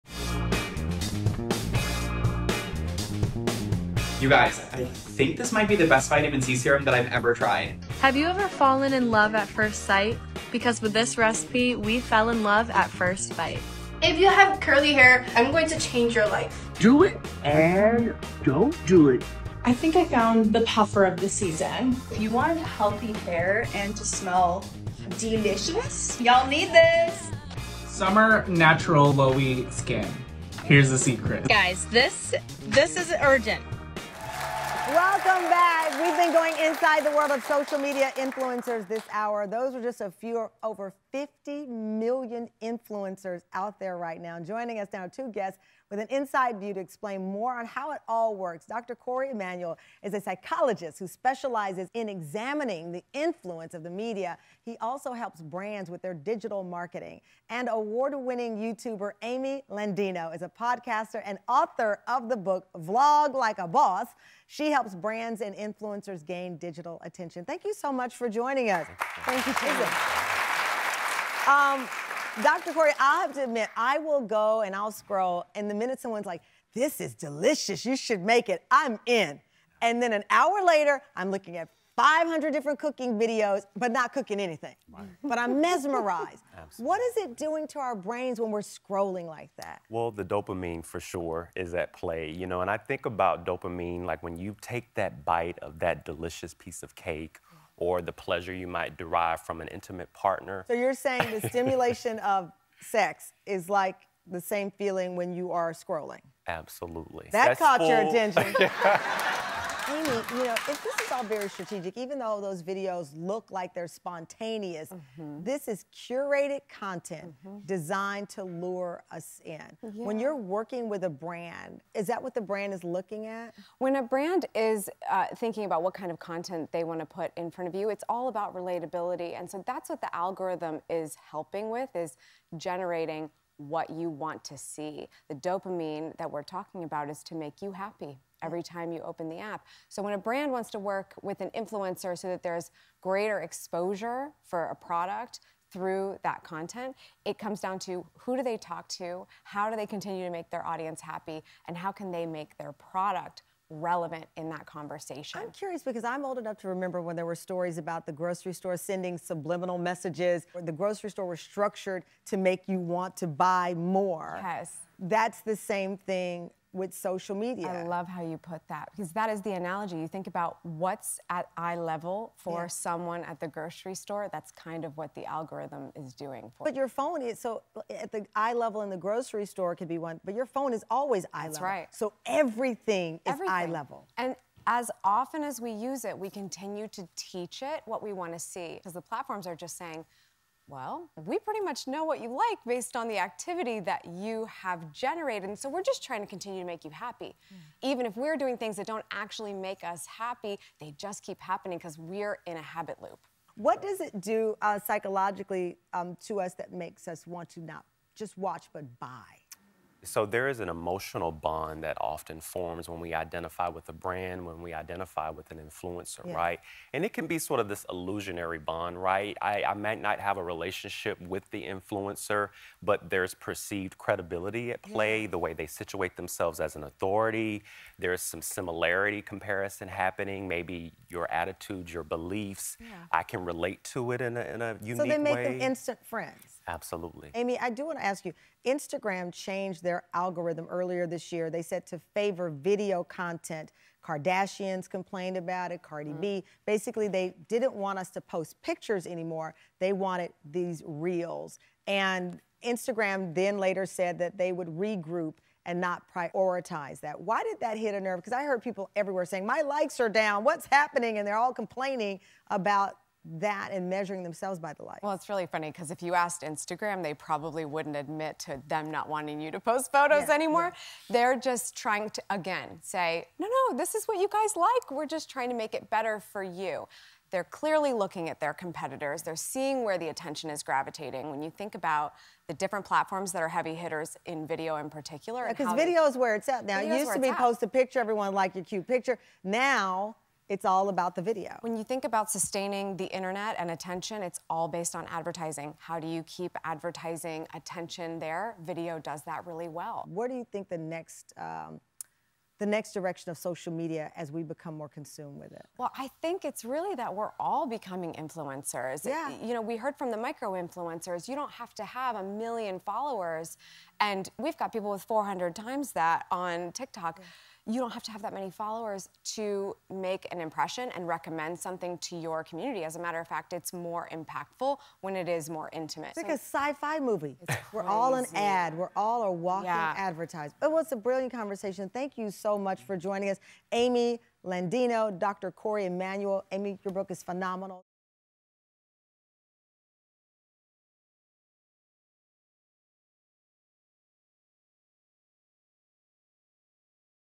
Here’s What the Stimulation Of Scrolling on Social Media Does to Your Brain
Dr. Corey Emanuel, a media phycologist who examines the impact of social media on society, and influencer brand consultant Amy Landino join our episode titled, “Are You Influenced Yet?”. The social media experts explore the effects that social media platforms have on our daily lives and beyond. It’s a thought-provoking sit-down that you won’t be able to stop talking about.